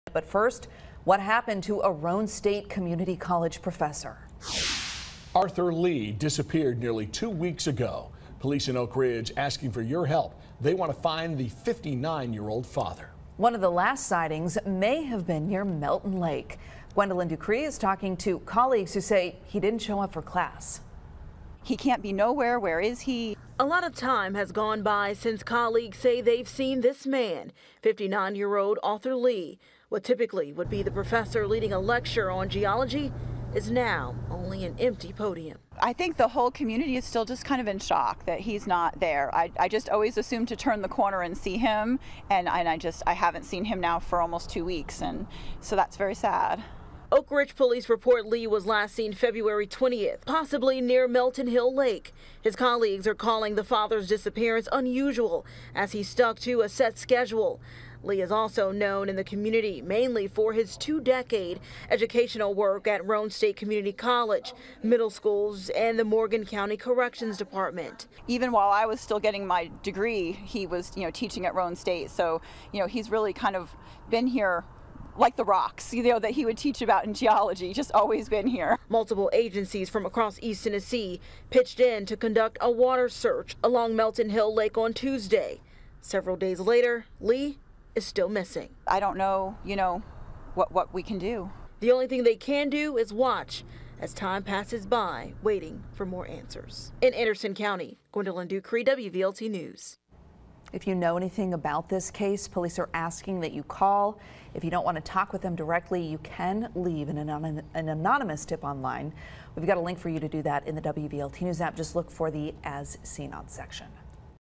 Disappearance of Roane State professor stuns colleagues
One of the last sightings of the professor might have been near Melton Lake.

Police are asking anyone to give them a call or leave an anonymous tip if they know where Arthur Lee could be.